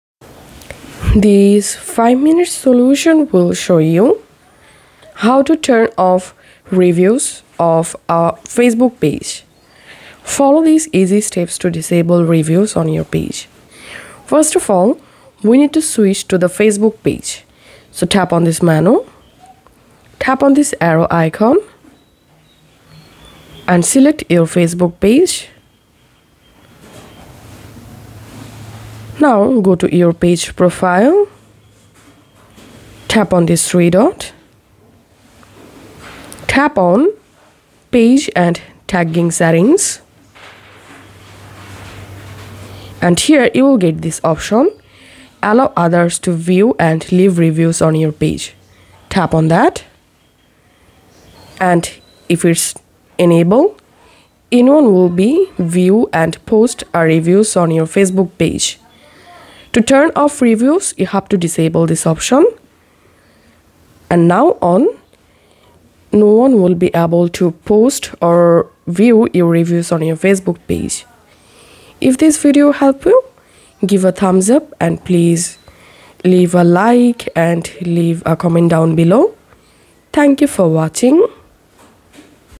How To Turn Off Reviews On A Facebook page - Full Guide
Your business might hit a rough patch, but this too shall pass. Learn how to turn off Facebook reviews to help you ride those tough times out.